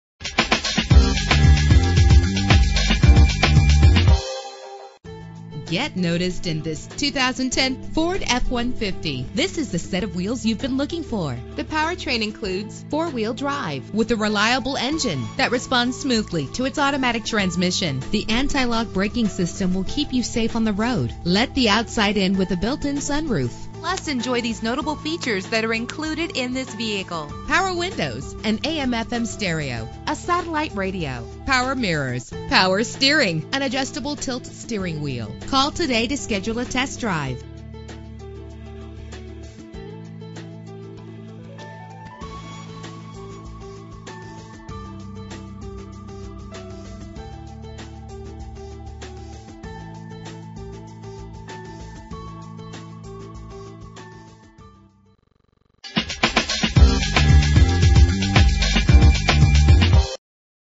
2010 Ford F-150 - Cicero NY
Year: 2010
Make: Ford
Model: F-150
Trim:
Engine: 5.4 L, 8 cylinder
Transmission:
Color: Red
Mileage: 49528
Address:
5885 E Circle Dr